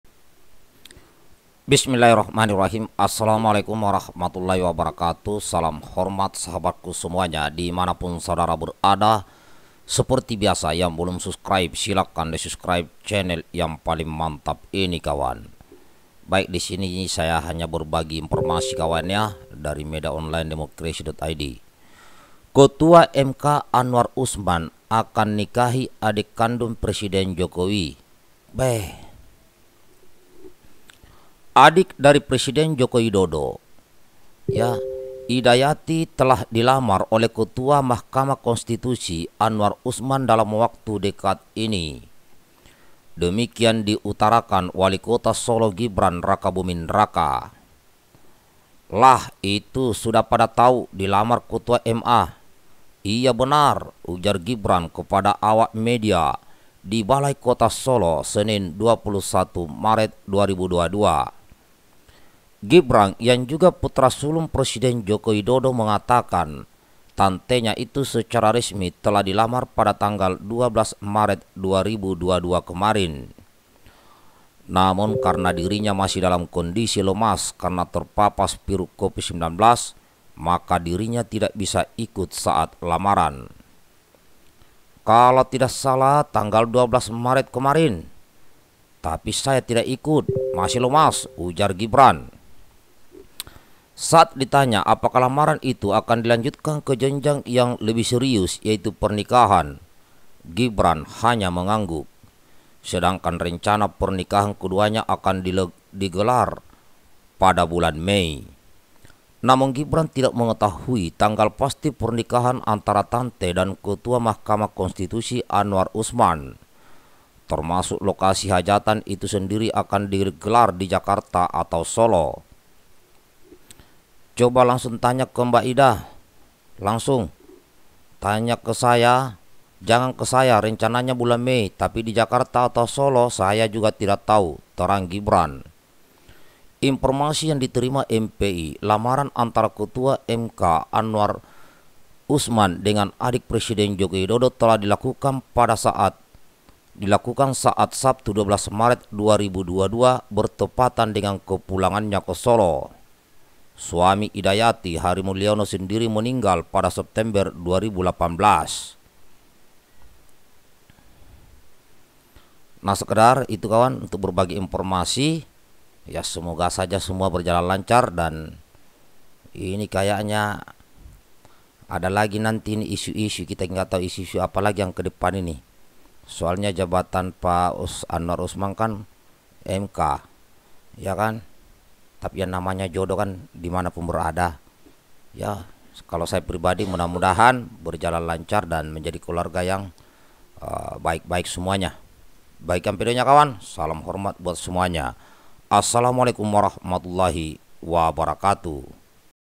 VIRAL. Ketua MK Anwar Usman Akan Nikahi Adik Presiden Jokowi. MPS Sampaikan Pesan
Aamiin.

TTD